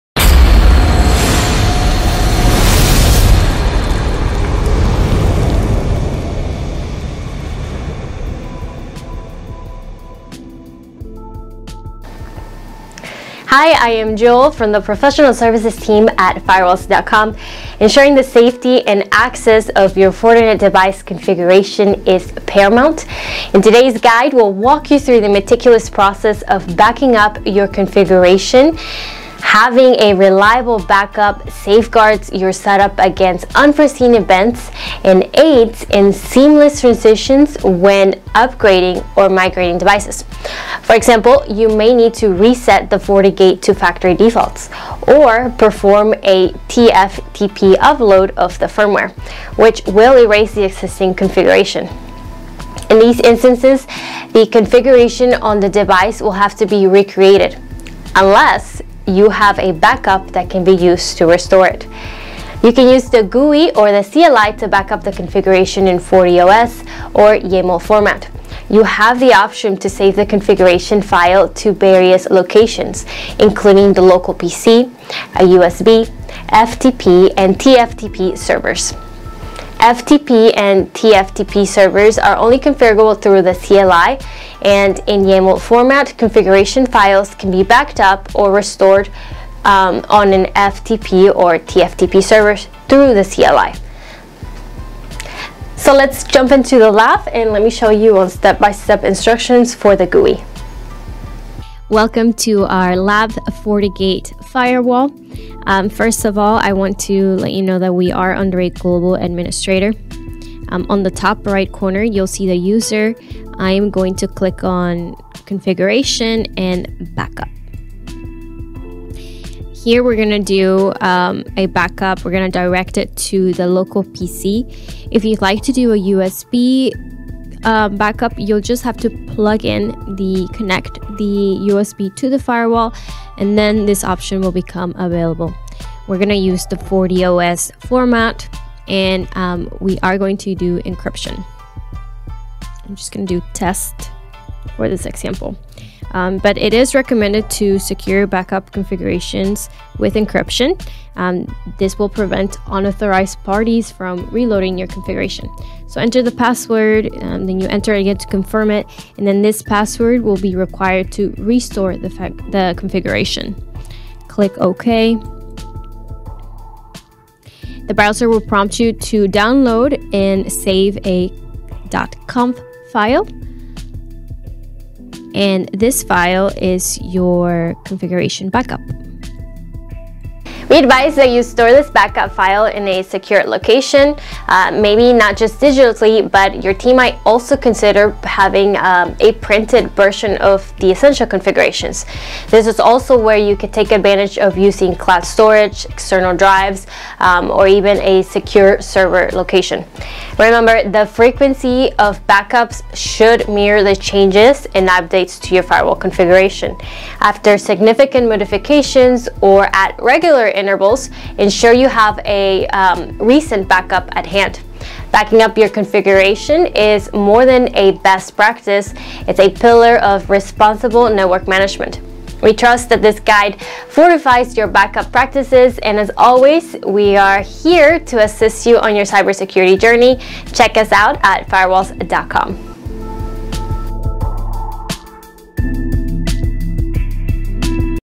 Fortinet: How to Back Up Configuration on your FortiGate Firewall
In this video, Network Engineer Jo walks you through the process of backing up your configuration on your Fortinet device. Having a reliable backup protects your setup against unforeseen events and makes upgrades and migrations easier.